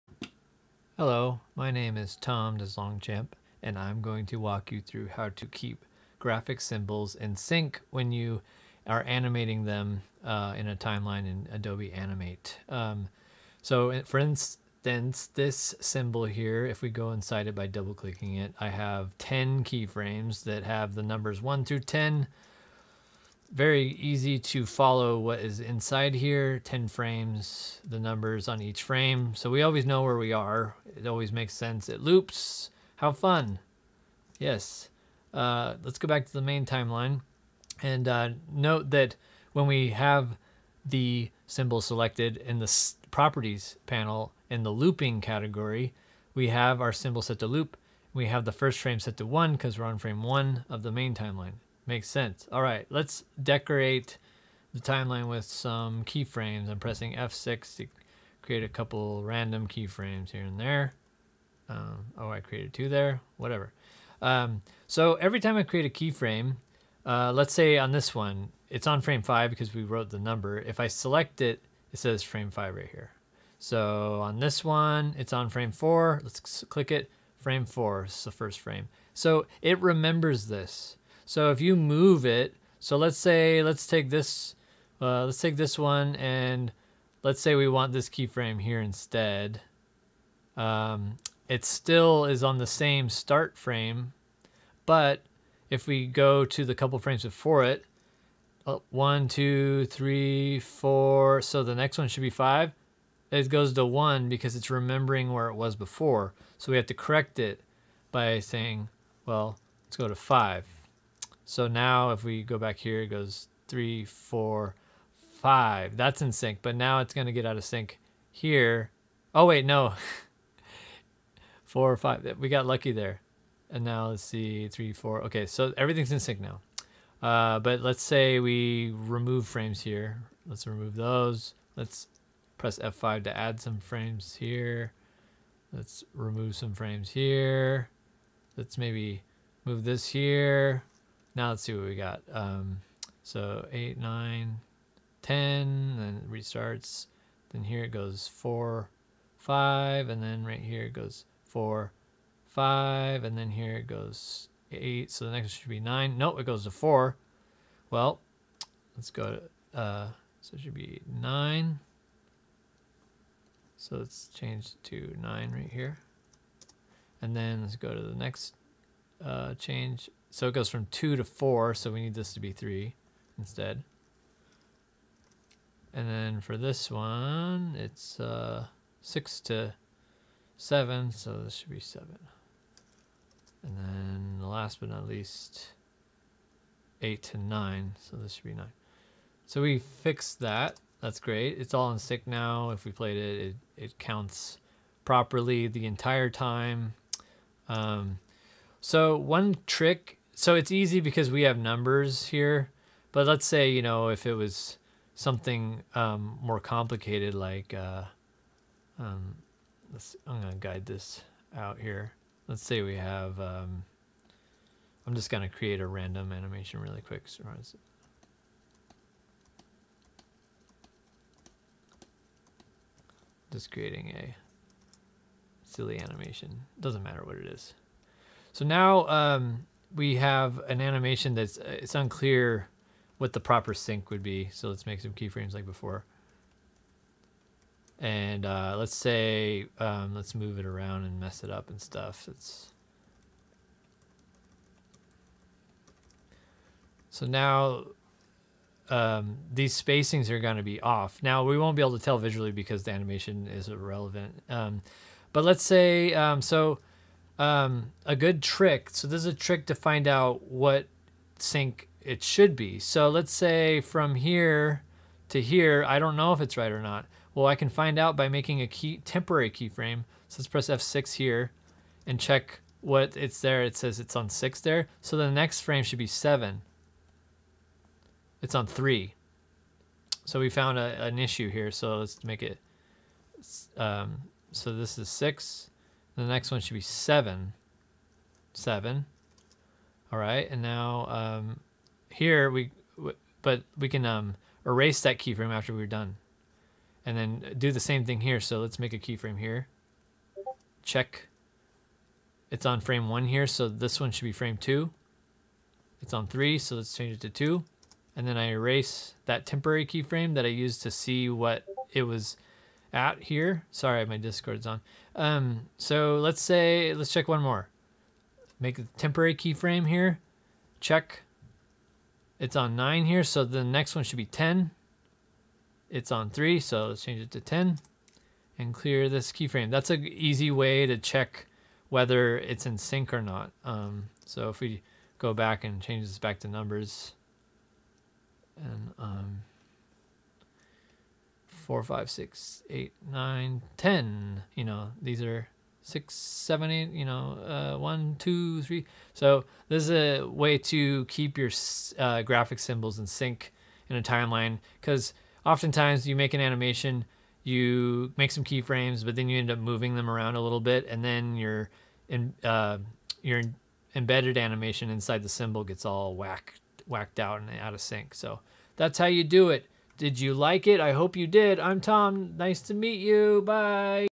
Adobe Animate: Keeping Graphic Symbols in Sync
Hi. I'm Tom DesLongchamp. I've been working in Flash/Animate for 20 years. I want to share some helpful tips like this one: how to keep your symbol loops in sync on the parent timeline. As you create new keyframes and move them around, things can get off sync very easily.  I show a good way to fix sync issues.